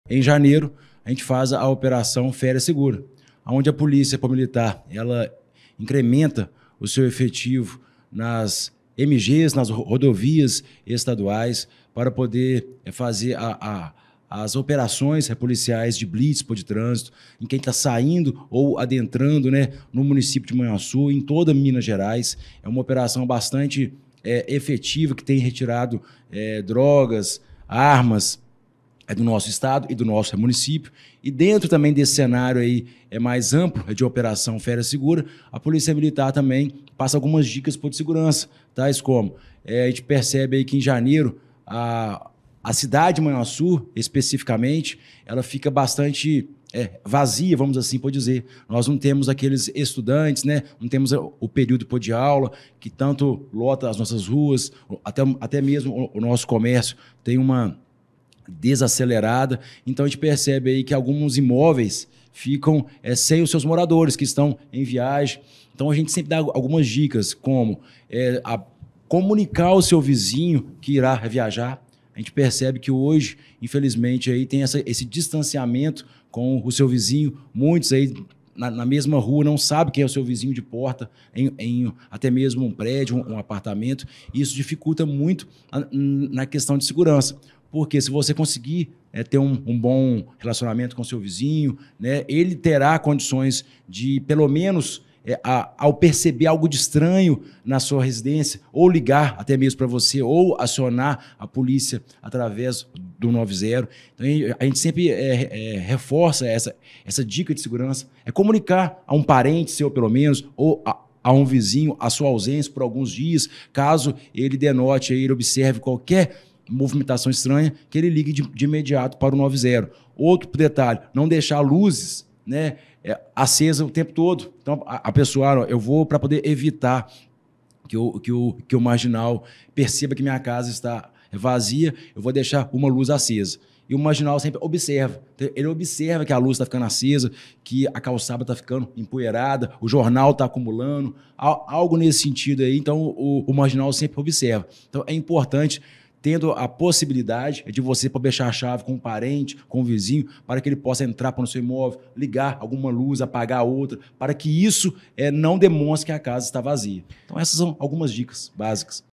PMMG inicia operação Férias Seguras para combater a criminalidade
A Polícia Militar de Minas Gerais (PMMG) iniciou a operação "Férias Seguras" com o objetivo de prevenir acidentes de trânsito durante o período de férias, quando há um aumento significativo no tráfego. A ação também visa combater a criminalidade, concentrando esforços na apreensão de armas de fogo irregulares, na prisão de foragidos da justiça e na repressão ao tráfico de drogas. A operação busca garantir a segurança dos cidadãos durante as férias, contribuindo para a redução de incidentes no trânsito e a promoção da ordem pública.